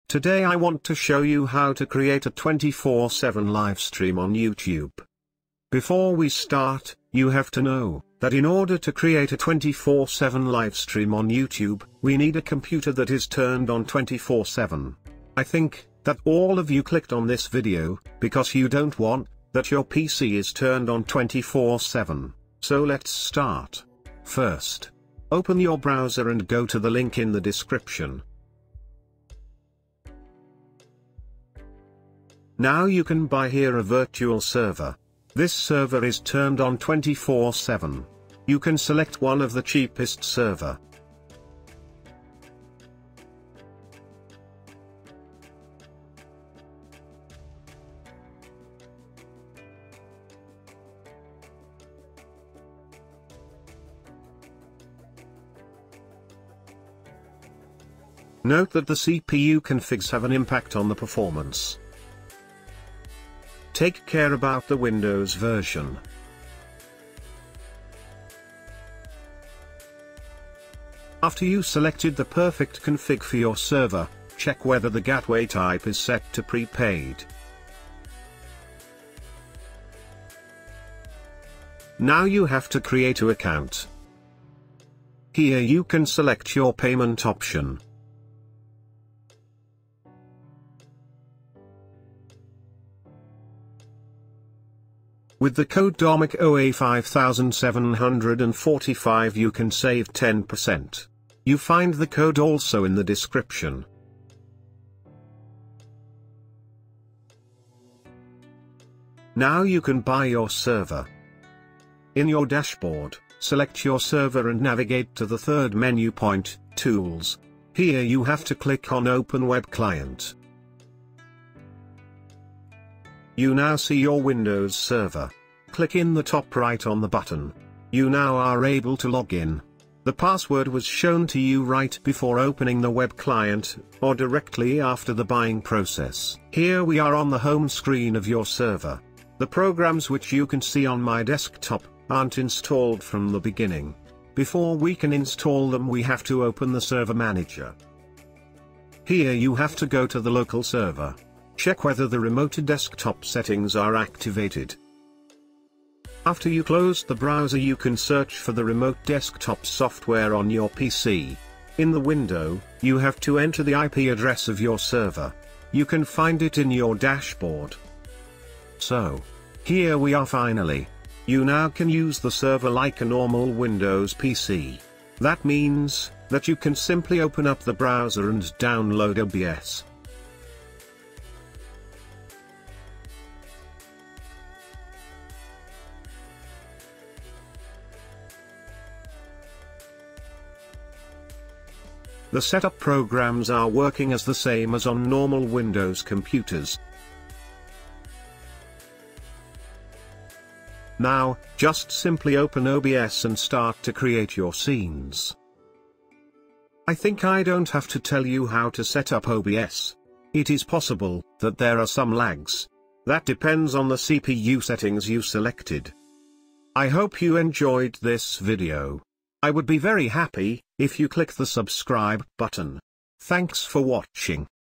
🔴 HOW TO Create a 24/7 Live Stream on YouTube WITHOUT running your PC 24/7!
HOW TO Make a 24/7  Live Stream on YouTube or Twitch WITHOUT running your PC 24/7!

Today I want to show you how to make a 24/7 Livestream on YouTube without running your PC 24/7.
⚠️⚠️ Important❗ Pay attention to the CPU settings. The ones in my video are NOT made for 720p streams! No warranty! ❗❗❗
Save 20% Code: Domiko-a-5745
(Of course, it is possible to use another server hoster! I use Zap-Hosting because I have good experiences with them.)